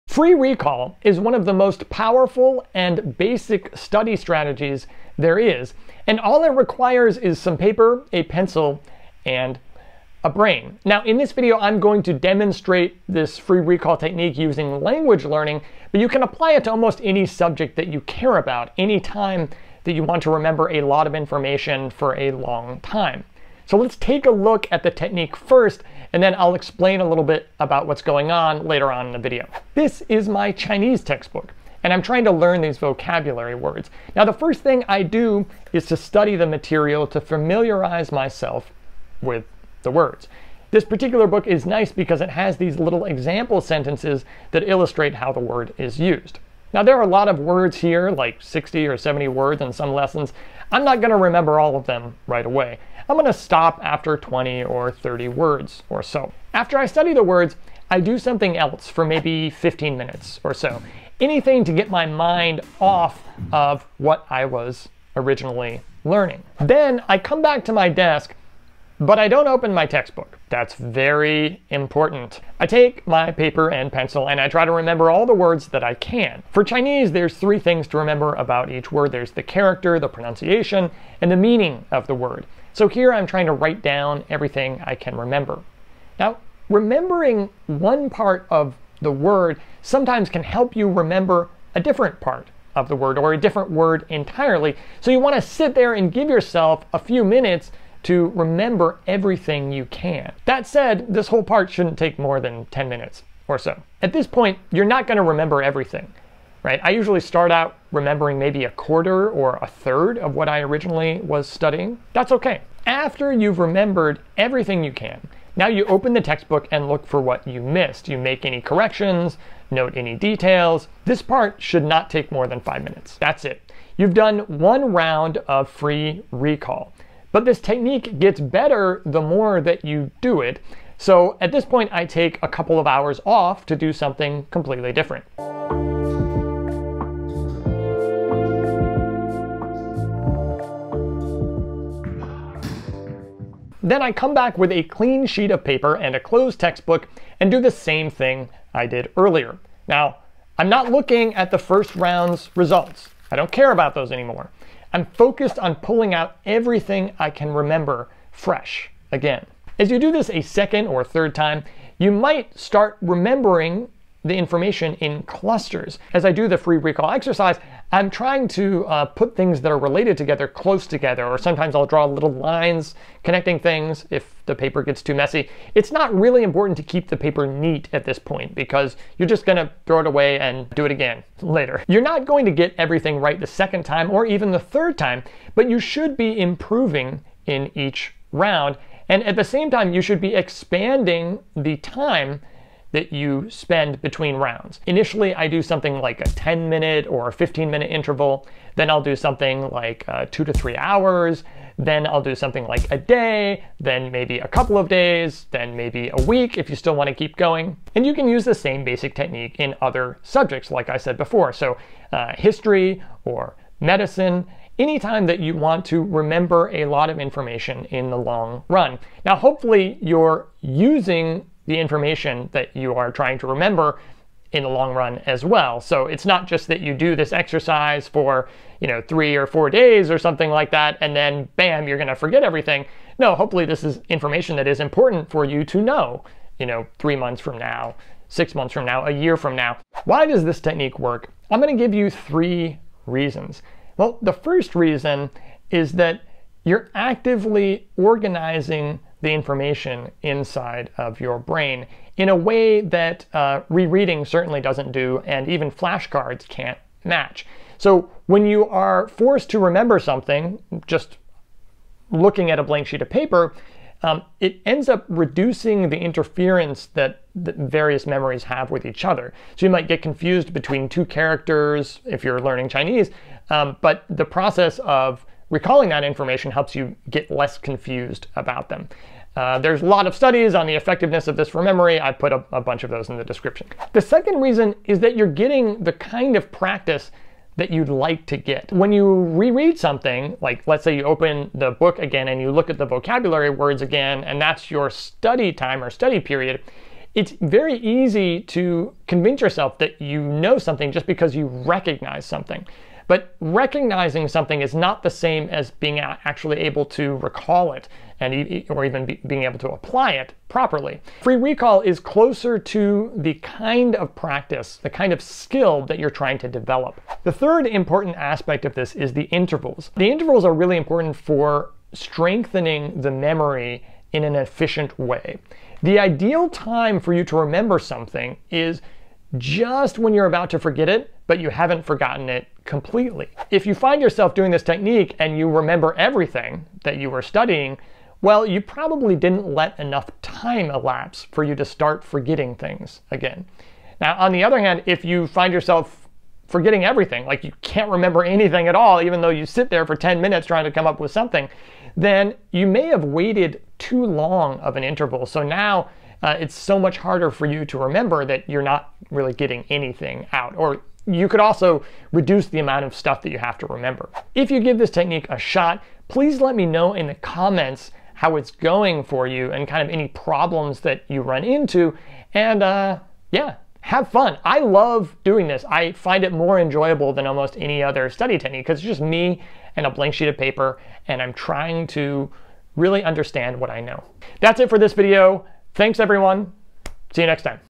How to do free recall (AKA active recall) - Language learning demonstration
Free or active recall is one of the simplest and most effective ways of studying. You can use it for language learning, learning anatomy, learning history - anytime you want to remember information in the long-term. But many students are either unaware of free recall or don't know how to do it very well.

In this video, I demonstrate free recall and explain a little bit about why it works.

00:00 Introduction
00:29 A demonstration of free recall.
2:07 Round two.
3:14 Recommended intervals.
4:00 Why does free recall work?
5:33 Troubleshooting the technique.

References:

On the superiority of free recall to elaboration, see:

Karpicke, J. D., & Blunt, J. R. (2011). Retrieval practice produces more learning than elaborative studying with concept mapping. Science, 331(6018), 772-775.

Karpicke, J. D., & Roediger III, H. L. (2008). The critical importance of retrieval for learning. science, 319(5865), 966-968.

Free recall is most effective only when the items have been "cleared" from your short term memory.

Karpicke, J. D., & Roediger III, H. L. (2007). Repeated retrieval during learning is the key to long-term retention. Journal of Memory and Language, 57(2), 151-162.

On optimal spacing intervals, see:
Carpenter, S. K, Cepeda, N. J., Rohrer, D., Kang, S. H. K., & Pashler, H. (2012). Using spacing to enhance diverse forms of learning: Review of recent research and implications for instruction. Educational Psychology Review, 24, 369-378.

Free recall "enhances organizational processes". In other words, it reduces interferences and helps us to organize what we know.

Zaromb, F. M., & Roediger, H. L. (2010). The testing effect in free recall is associated with enhanced organizational processes. Memory & cognition, 38(8), 995-1008.